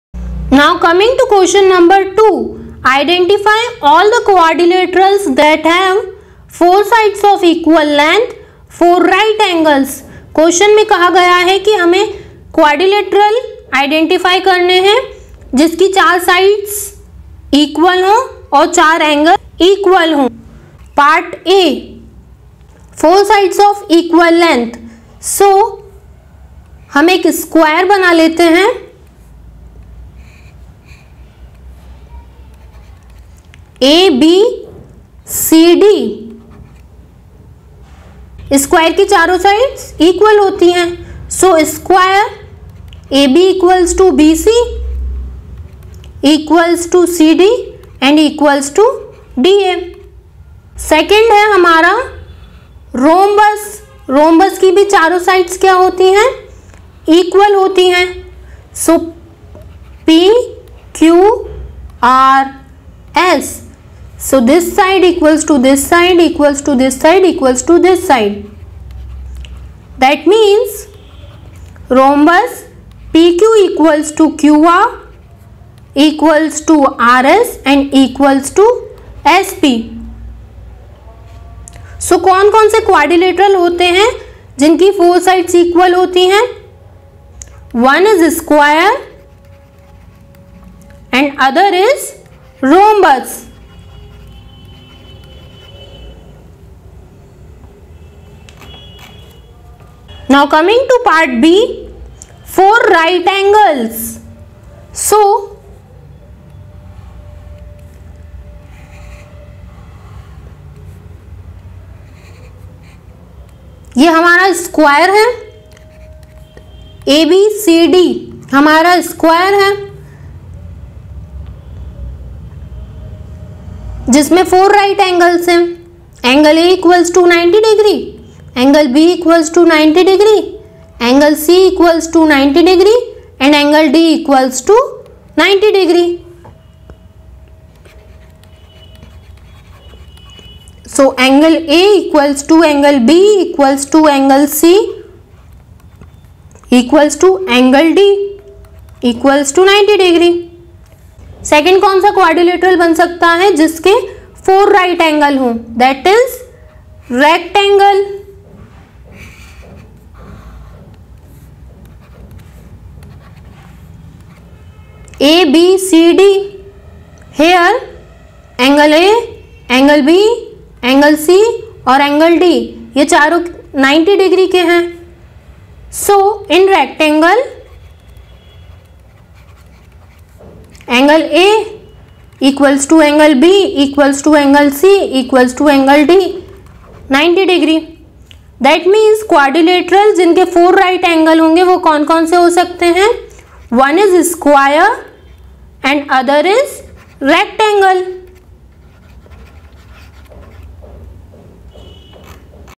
2. Identify all the quadrilaterals that have (a) four sides of equal length (b) four right angles
Identify all the quadrilaterals that have (a) four sides of equal length (b) four right angles